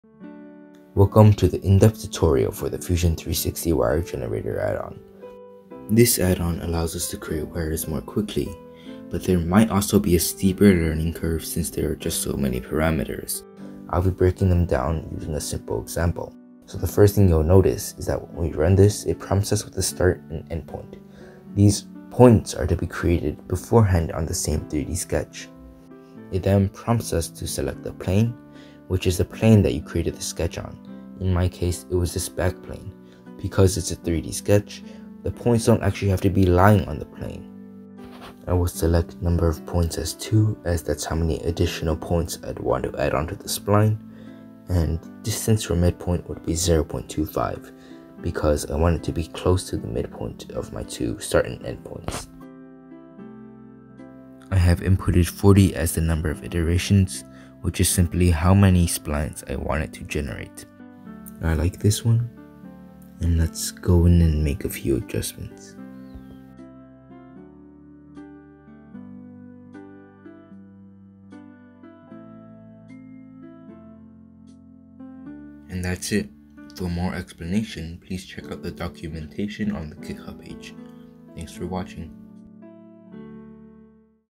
Making Wires in Fusion 360: Wire Generator Add-On Tutorial
DOWNLOAD

DESCRIPTION
This Fusion360 Add-on helps us create wires. You can find the script onto your computer by going to the Github page and installing it from there.

With this tool, simply select your start and end points, input your parameters, and the program generates several spline variations. Select the right one for your fit, and easily adjust by changing the spline points.

OTHER PARAMETERS

- Number of Points: Number of points to be generated other than selected start-end points
- Wire Radius: Radius of wire that is going to be generated
- Spread: The distribution of the points that are going to be generated for a spline
- Iterations: Number of spline variations generated
- Angle: Adjust the angle of the wire generated
- Angle Variance: Deviation of the angle. Angle Range around the angle.
- Distance from Midpoint: Perpendicular distance of the generated spline points from the midpoint of the start - end line.
- Disable Sweep: Activating Disable sweep will only generate the spline and not perform the sweep path motion that creates the body